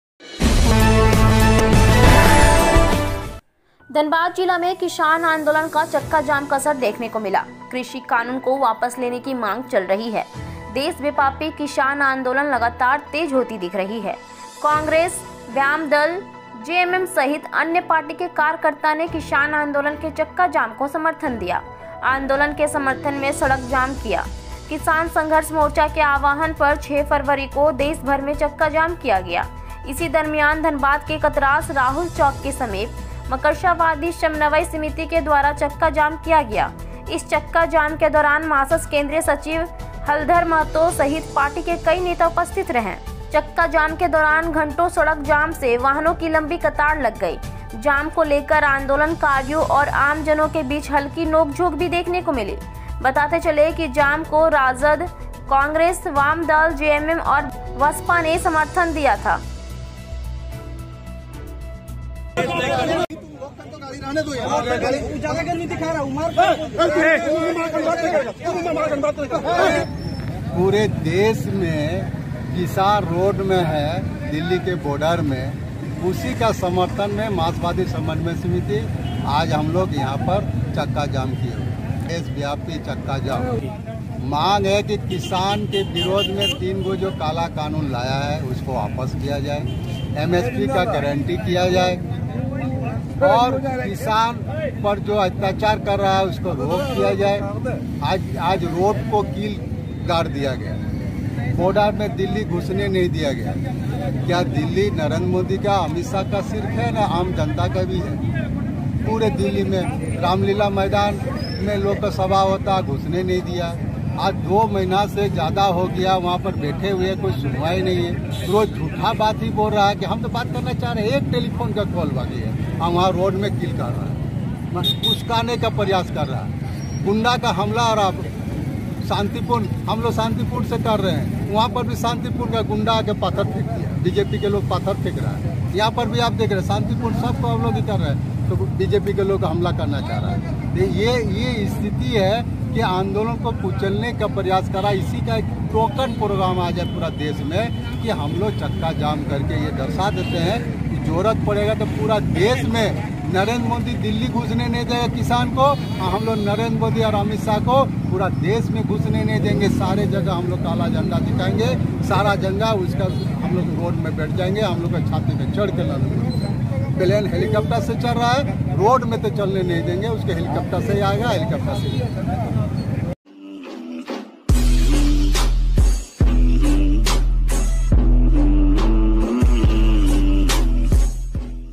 किसान आंदोलन के देशव्यापी चक्का जाम के समर्थन में किया गया सड़क जाम
बिहार और झारखंड से जुड़ी खबर देखने के लिए जुड़े रहिए हमारे साथ ।